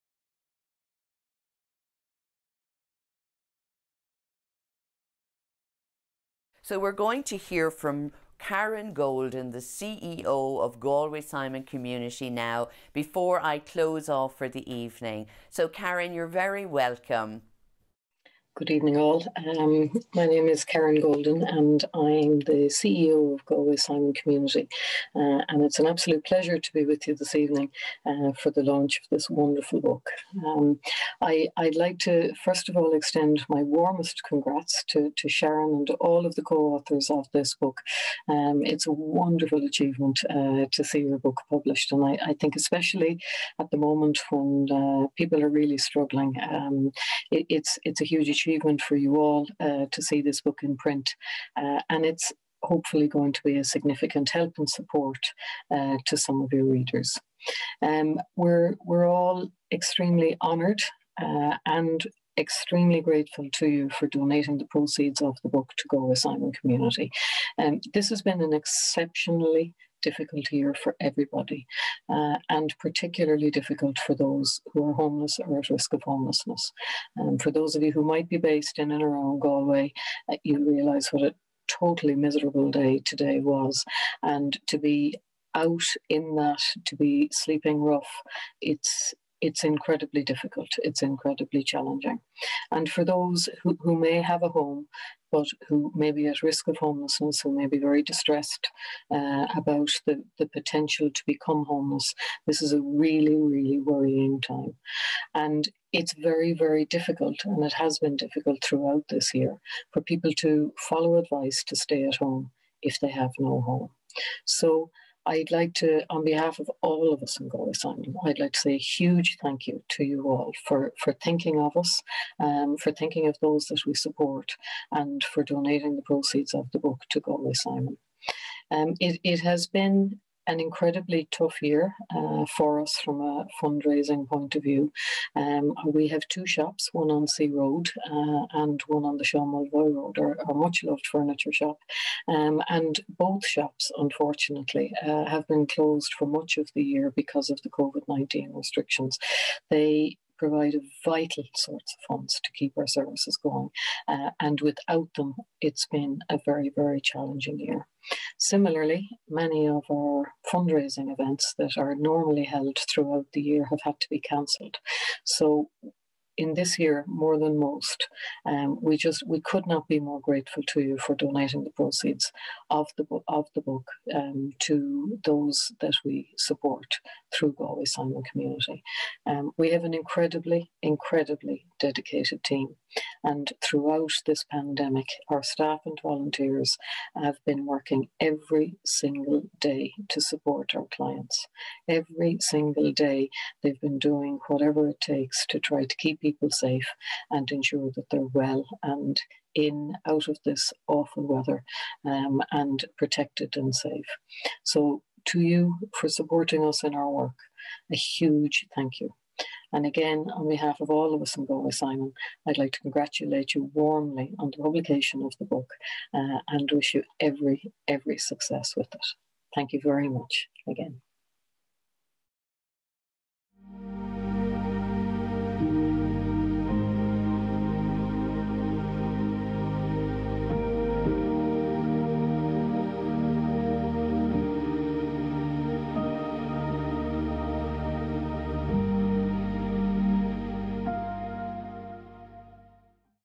Karen Golden Galway Simon - Awaken Your Wellbeing 2020
Part of the official book launch of Awaken Your Wellbeing. Live Stream Produced by Bright Blue Productions

All proceeds of this book go to Galway Simon Community in helping them support and prevent those affected by homelessness in the West of Ireland.